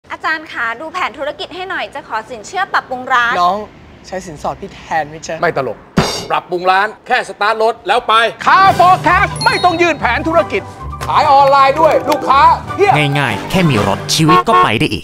คาร์ ฟอร์ แคช ง่าย ๆ แค่มีรถ ชีวิตก็ไปได้อีก
อยากขยายร้าน แค่มีรถ แล้วมา คาร์ ฟอร์ แคช
- ไม่ต้องยื่นแผนธุรกิจ
- กิจการแค่ 1 ปีก็กู้ได้
- ไม่ต้องกังวลเรื่องบัญชีรายได้